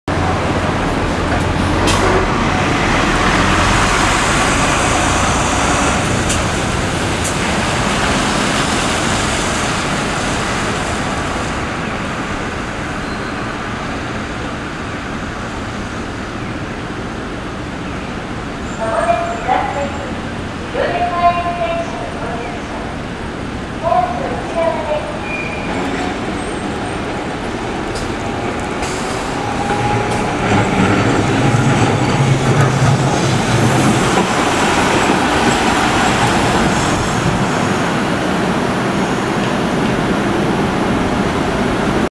広島電鉄900形 横川駅到着（列車接近放送付き）
雨のなか走る広電の900形です
元が、大阪市電の車両ですが、元気に今も走っています